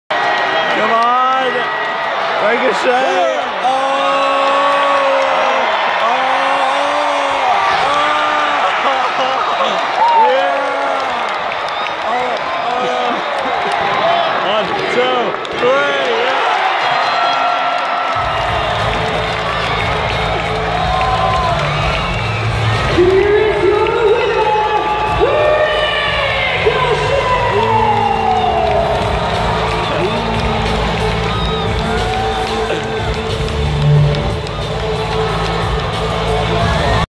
Ricochet puts Happy Corbin Through a table at WWE Ottawa
Happy Corbin vs Ricochet in a NO DQ match at WWE Ottawa from August 20th 2022